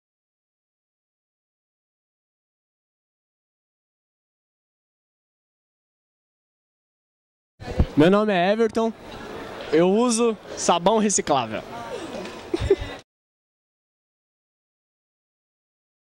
Depoimento Everton | Palanque Viva a Mata 2010
Depoimento coletado no evento Viva a Mata 2010, promovido pela Fundação SOS Mata Atlântica, entre os dias 21 e 23 de maio.

Os depoentes responderam à questão: o que você faz pela Mata Atlântica?